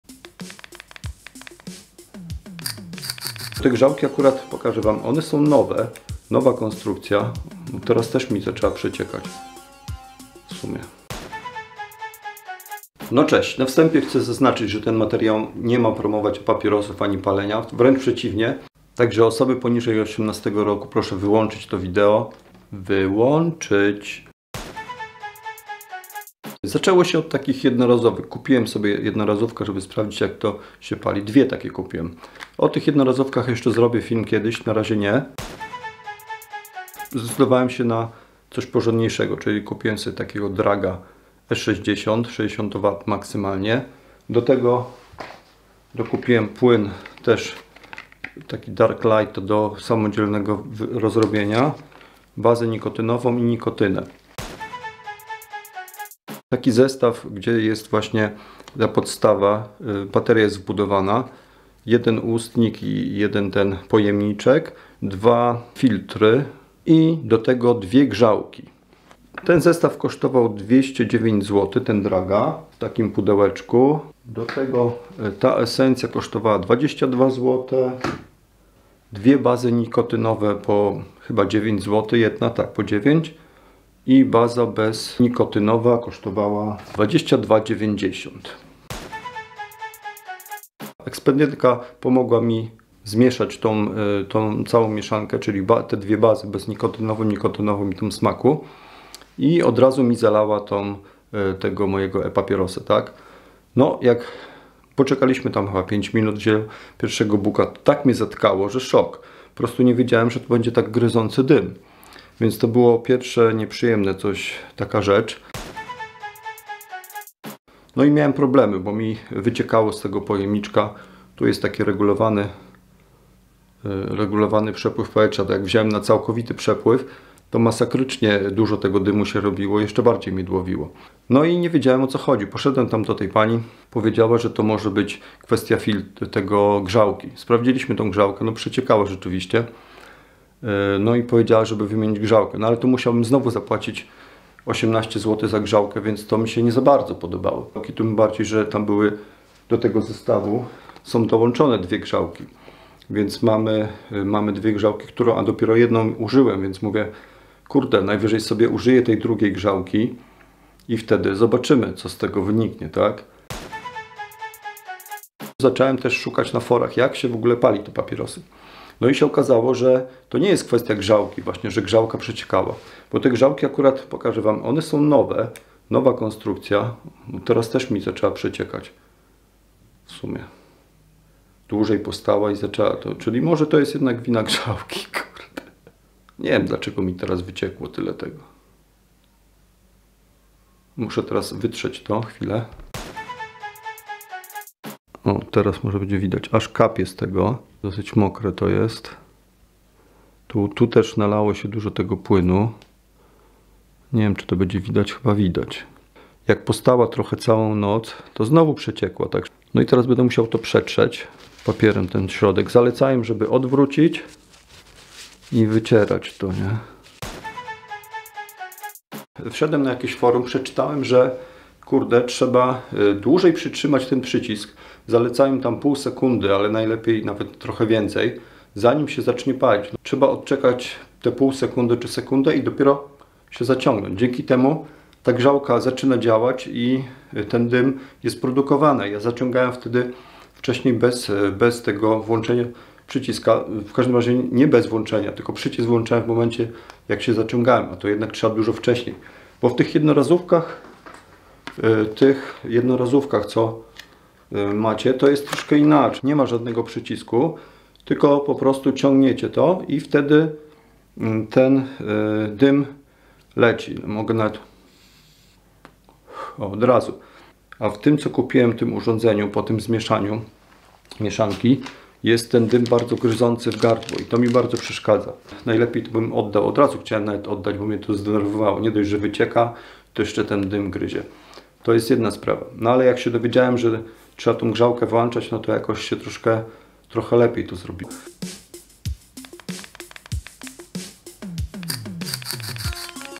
Pierwszy E Pet. co kupiłem? Problemy z Drag E60 VooPoo.
0:00 E-zajawka
0:11 E dla 18+
0:24 E-papieros Jednorazówka
0:35 Mój zestaw startowy
0:54 Cena E60 i dodatków
1:28 Pierwszy buch
1:55 Wyciekanie liquidu
2:45 Jak używać e-papierosa?
3:12 Osuszam z liquidu
3:39 Jak palić e-fajkę
4:48 Końcówka

Materiał jest wyłącznie edukacyjny, nie ma na celu promowanie palenia/vapowania, które jest przeznaczone dla osób dorosłych 18+ lat.